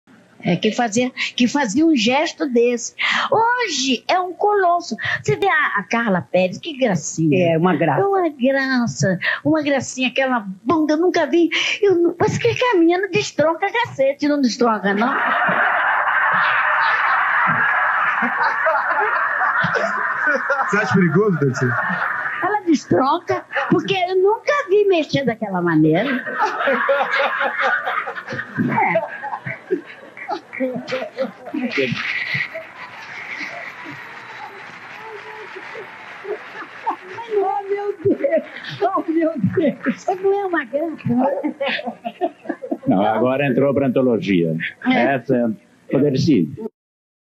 Dercy Gonçalves sobre a bunda da Carla Perez
Dercy sempre sincerona sobre a "bunda que destronca cacete"